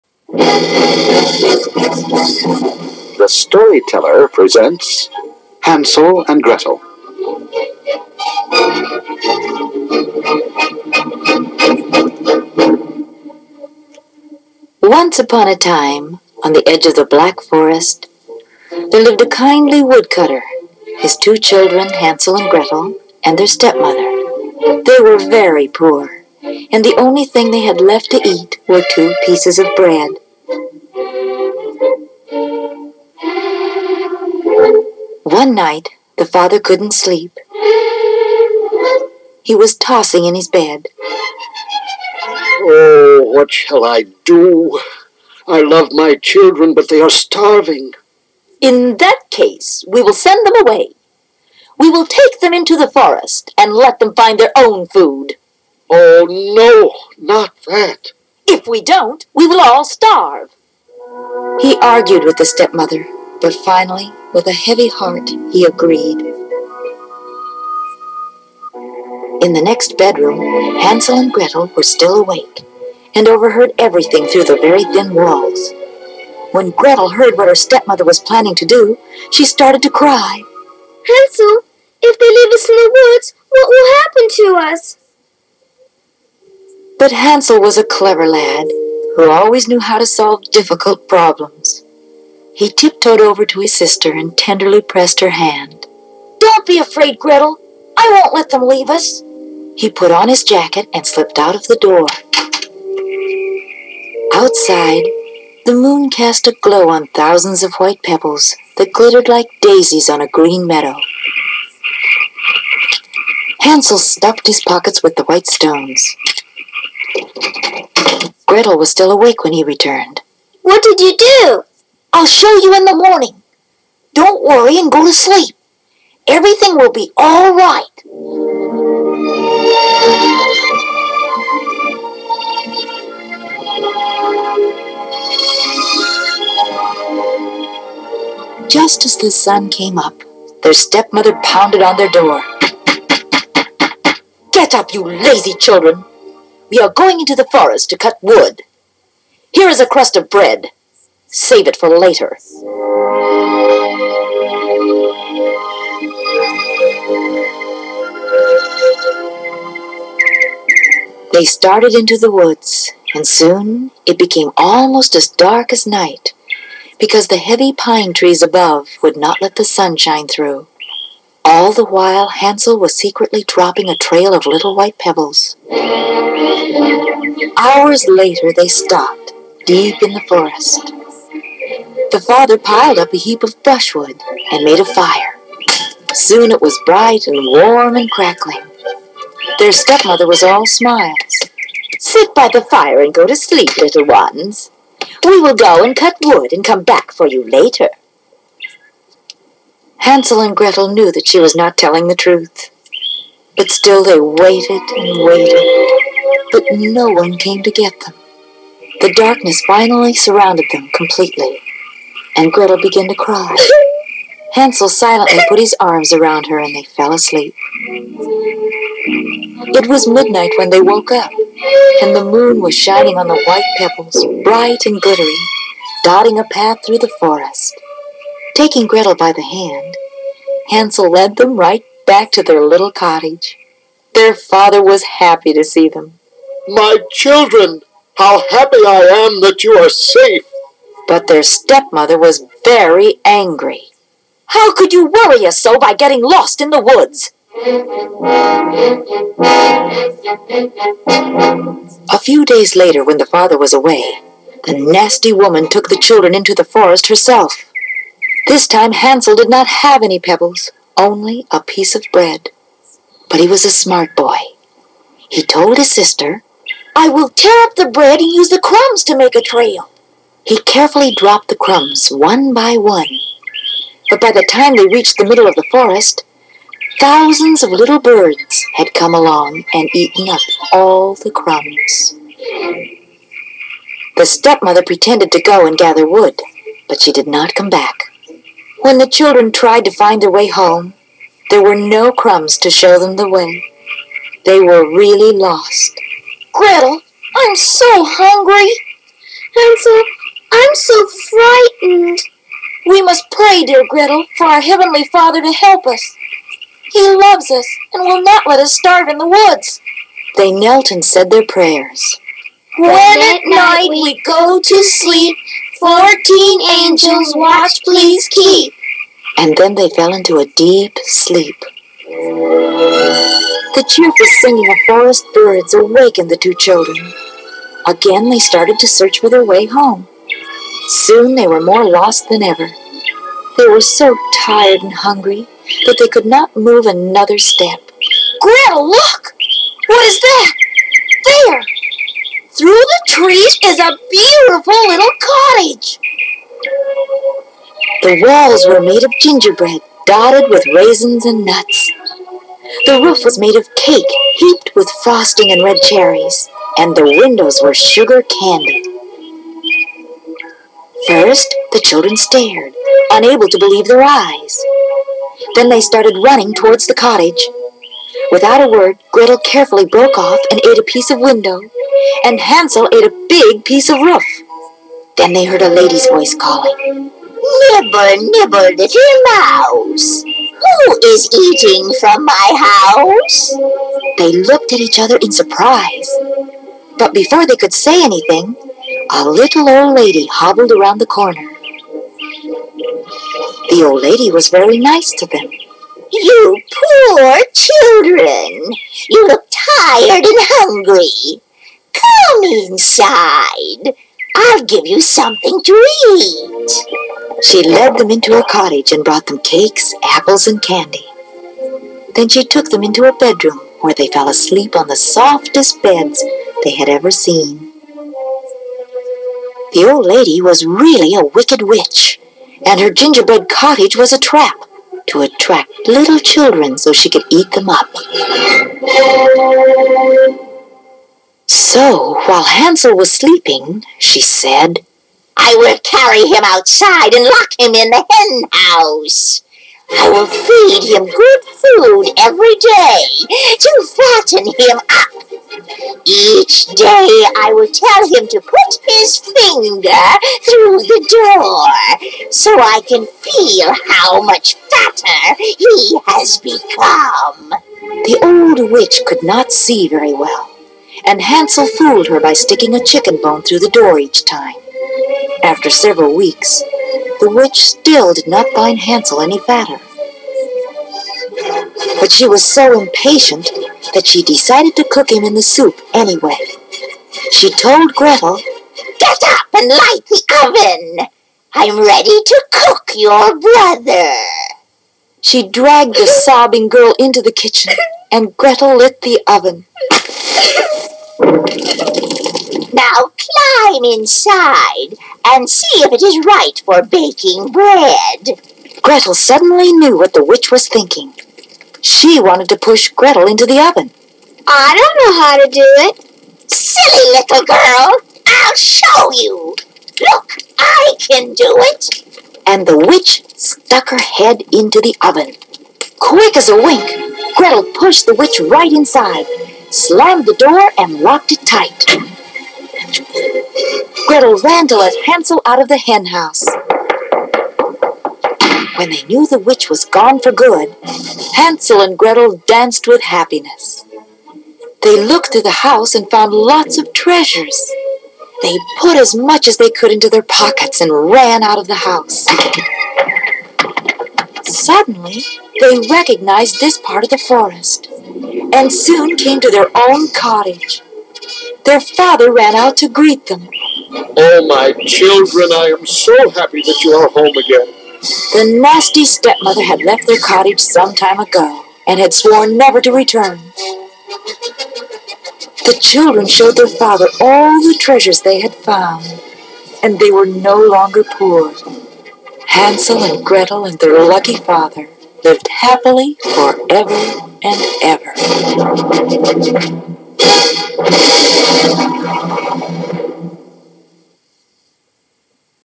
Hansel And Gretel The Story Teller
The Story Teller Presents Hansel And Gretel 1973. Design & Illustrations By Rex Irvine & John Strejan.